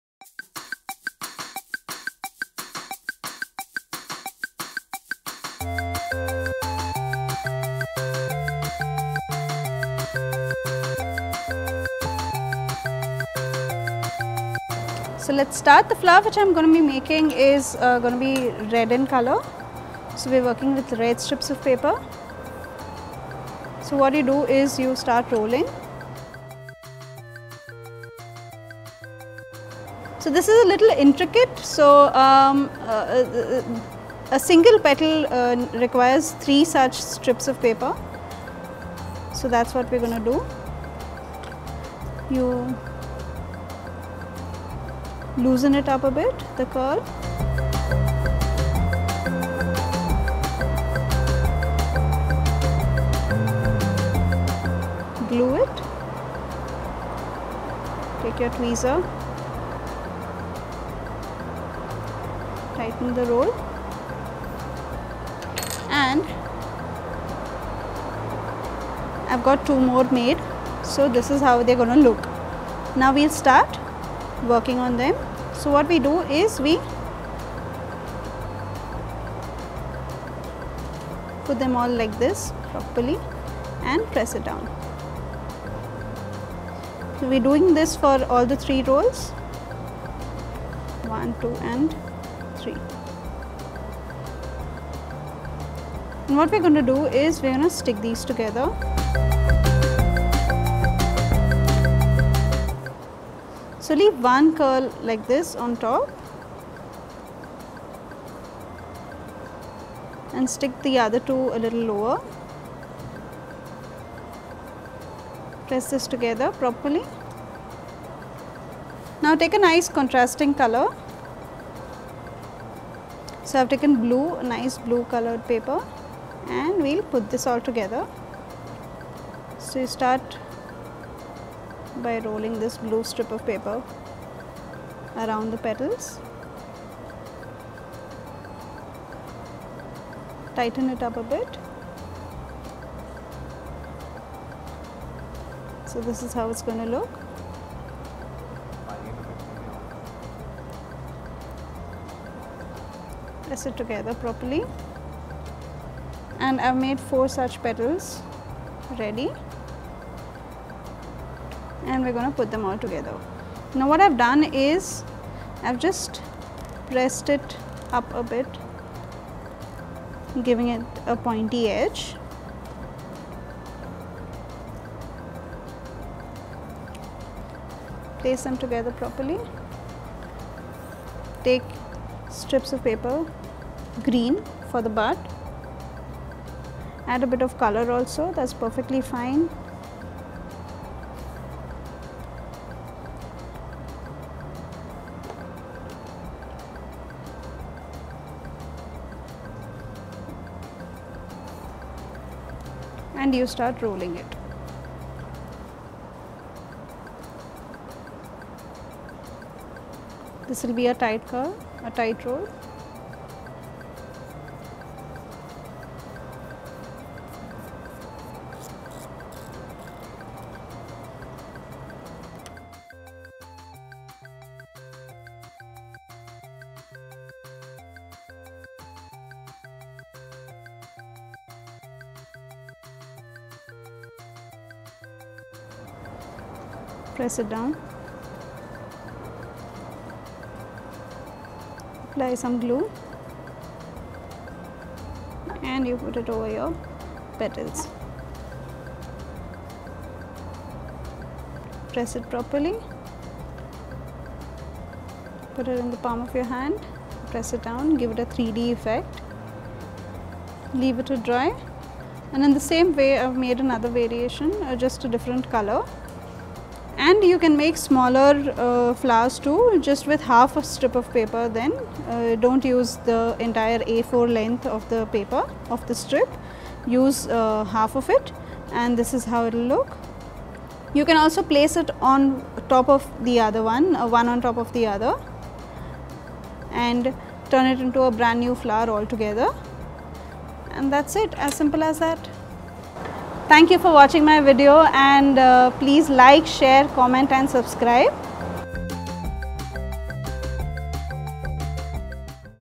How To Make Beautiful Flower Design Using Paper Art Quilling
Learn how to make beautiful quilling flowers with this detailed tutorial which can be used for greeting cards, bookmarks and as fridge magnets. So, lets get started!

Get crafty with PaperTales